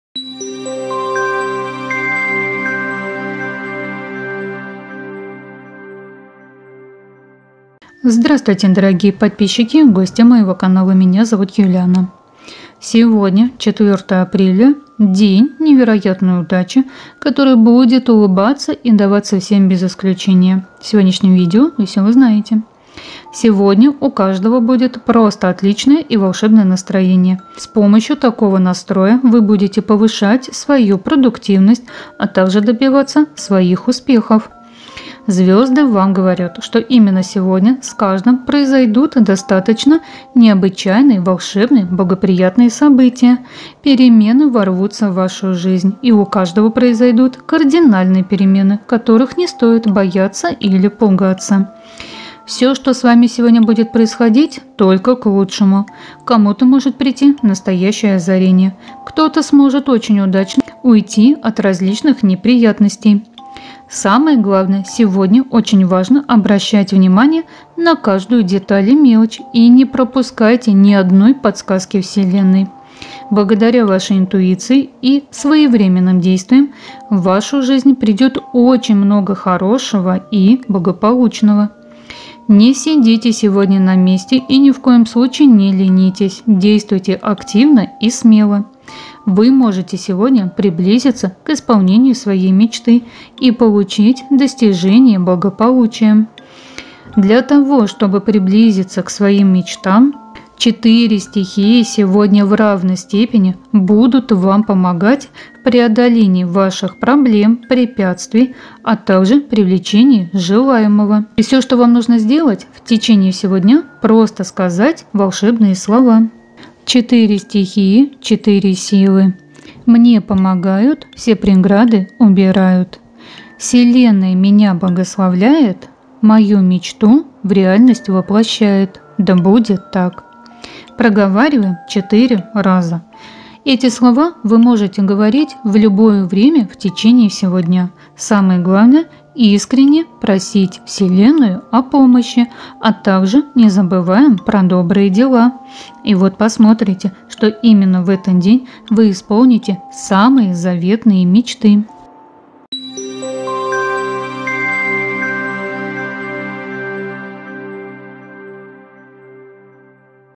Сегодня день невероятной удачи. Скажите волшебные слова.
Сегодня день невероятной удачи. Обязательно скажите волшебные слова.

Слова:

"Четыре стихии, четыре силы, мне помогают, все преграды убирают. Вселенная меня благословляет, мою мечту в реальность воплощает. Да будет так.

Благодарю Вас!

💰 Вы можете оказать поддержку проекту, любым удобным способом.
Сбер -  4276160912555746